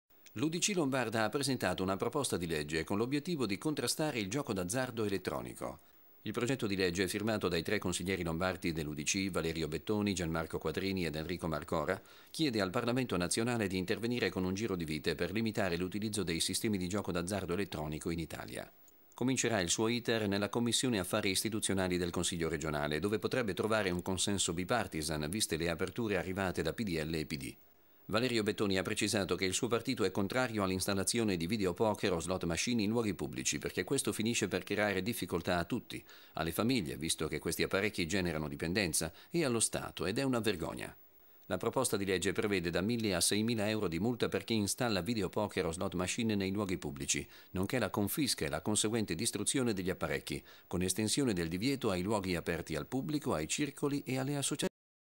UDC lombarda contro i videopoker Antenna 2 TV 301111.mpg
Iniziativa dell'Udc lombarda contro il gioco d'azzardo elettronico. Sentiamo Vladimir Lazzarini.